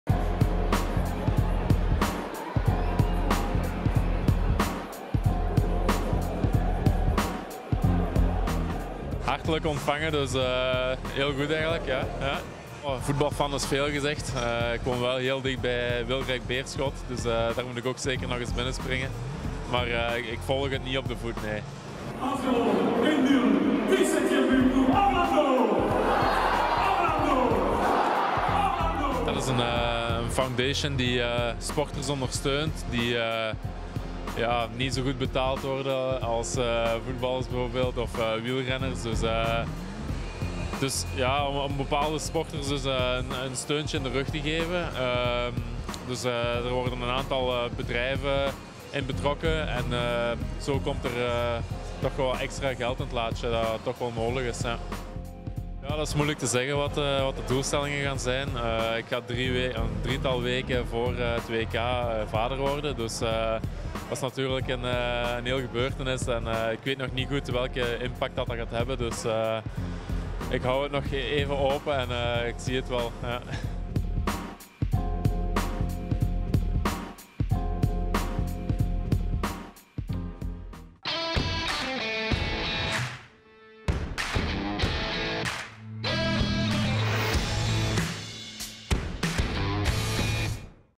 Pieter Timmers (médaillé d'argent au JO 2016) à Sclessin
Pieter Timmers, médaillé d’argent aux Jeux Olympiques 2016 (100 mètres nage libre), était invité à Sclessin pour présenter sa fondation BeStrong. Standard TV l’a rencontré pendant la mi-temps.

Pieter Timmers, zilveren medaillewinnaar op de Olympische Spelen van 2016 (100 meter vrije slag), was te gast op Sclessin om zijn foundation BeStrong voor te stellen. Standard TV ontmoette hem tijdens de rust.